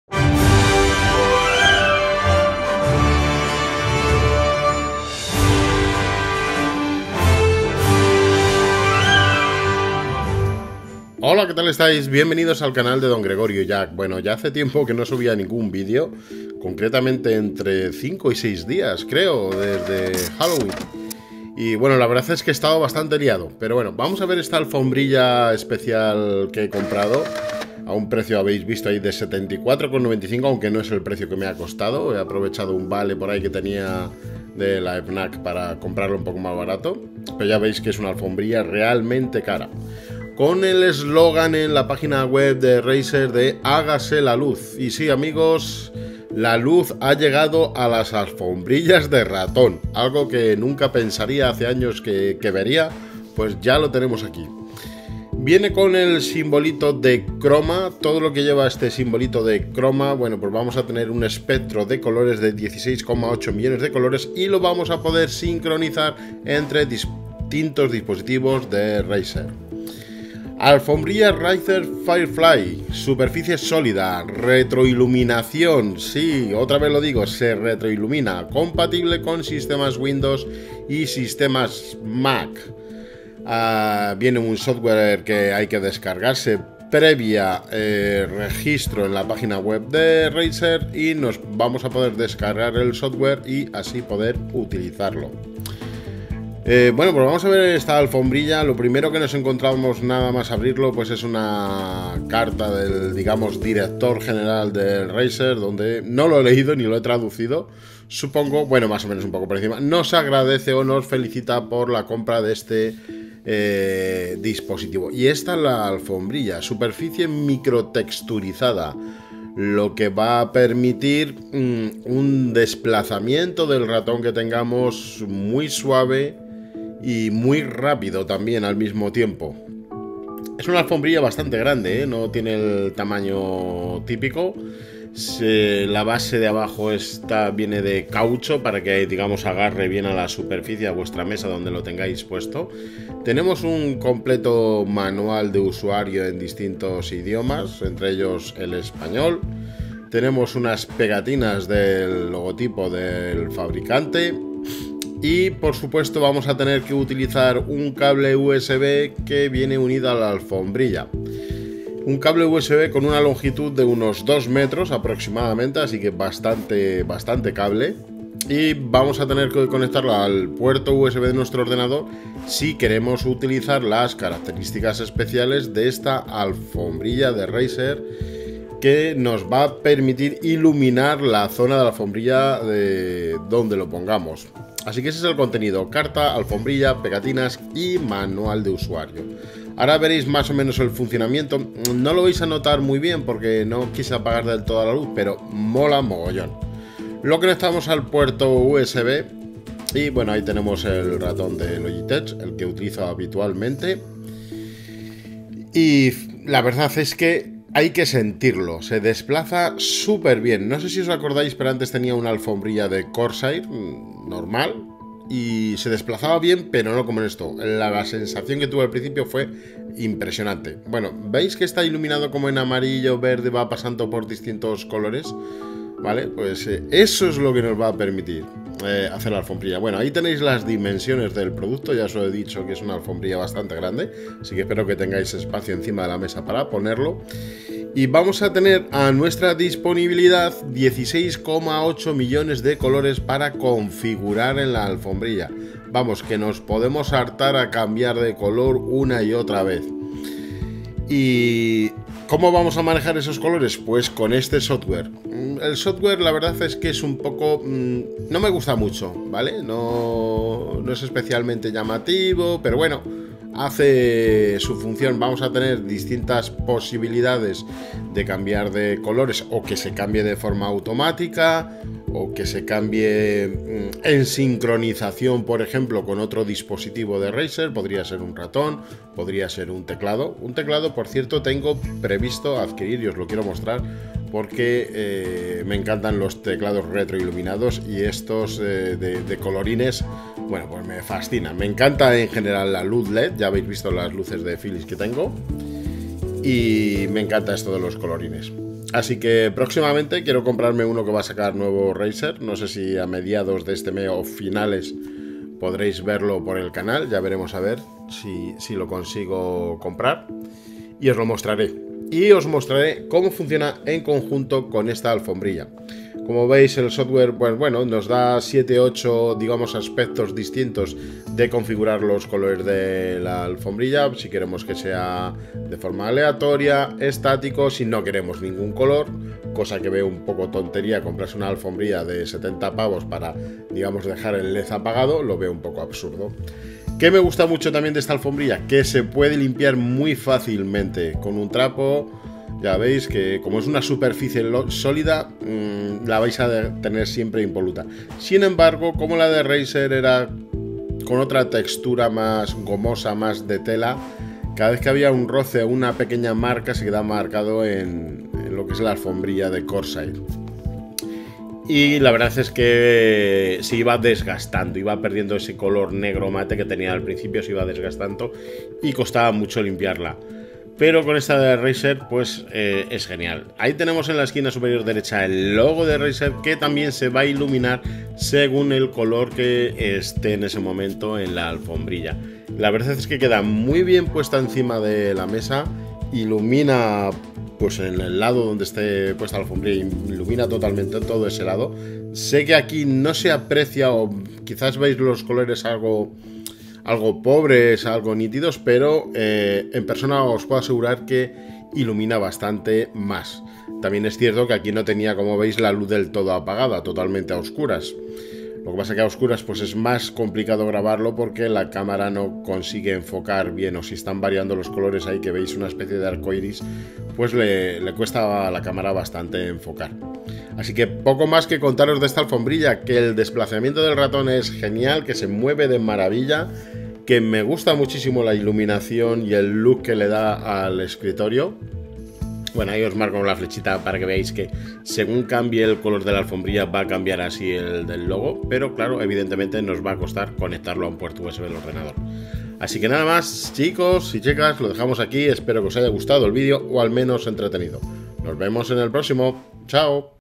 Alfombrilla de raton RAZER FIREFLY - en español
Hoy veremos en el canal una alfombrilla para nuestra raton algo especial. Es una alfombrilla retroiluminada, de grandes dimensiones y que se conectara a nuestro ordenador por medio de un cable usb.
Razer Firefly